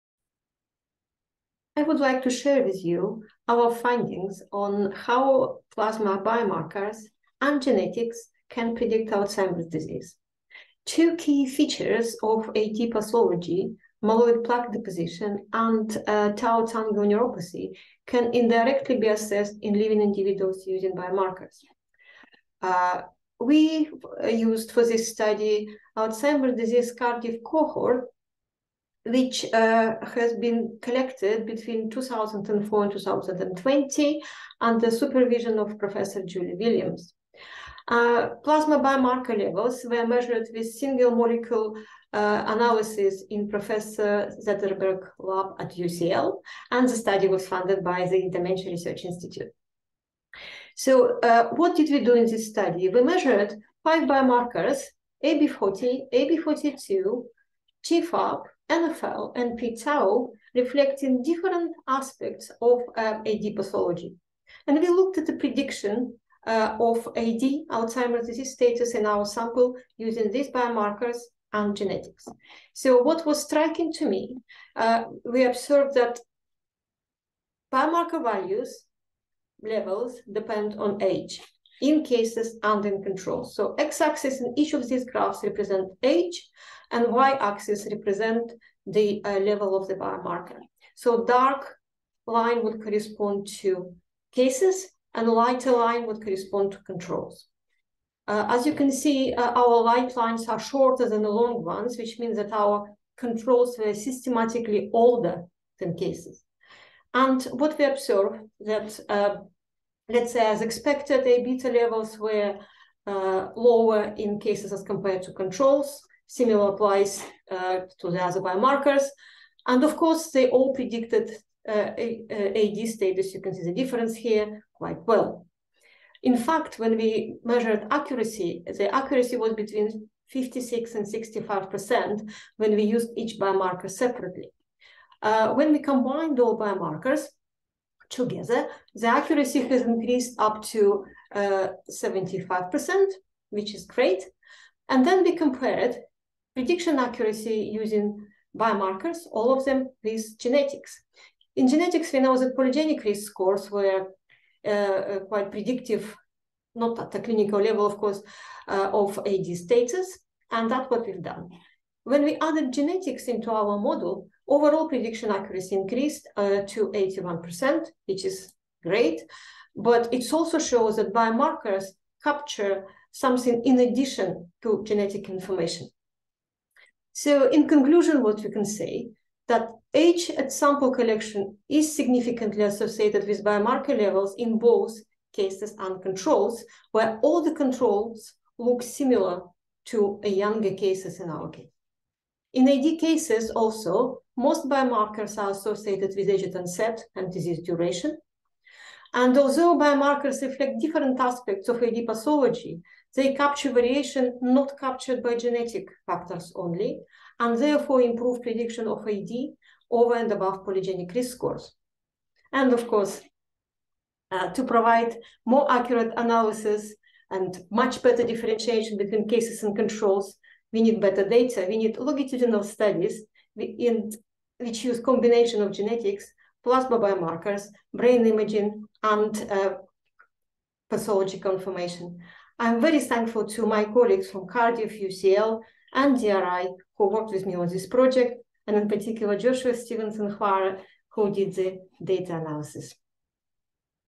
Plasma biomarkers and genetics in the diagnosis and prediction of Alzheimer’s disease
Stevenson-Hoare et al. show that five blood plasma biomarkers can together be used to diagnose and predict the onset of Alzheimer’s disease. Levels of the biomarkers are associated with age in both cases and controls, and show strong associations with genes including APOE-4.